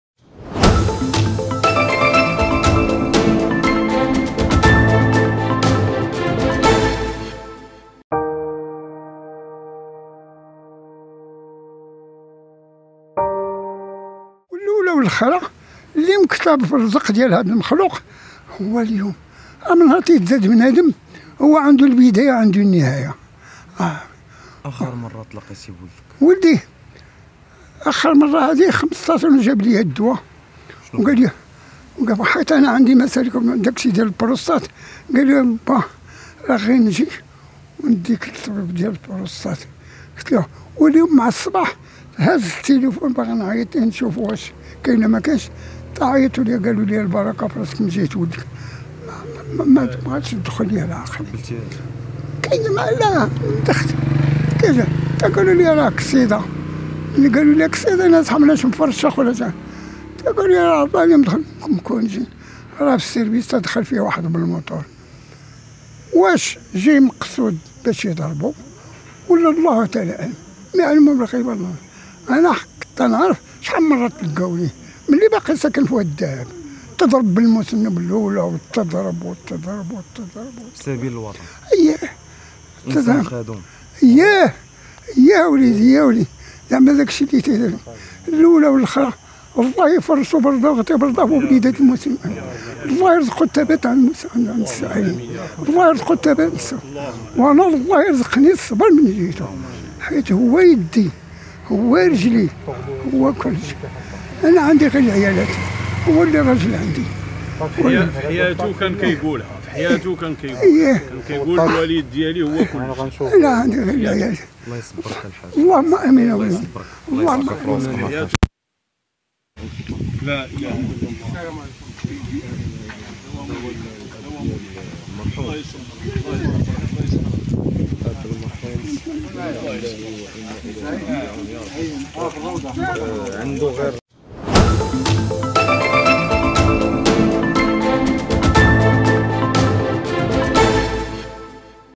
هذا ما قاله الشرطي ضحية حادثة سير لوالده قبل وفاته
بقلب حزين ودع والد الشرطي ضحية حادثة سير بالبيضاء إبنه. وفي هذا الفيديو ستقترب من تصريح الأب الذي أكد أن إبنه كان سنده في الحياة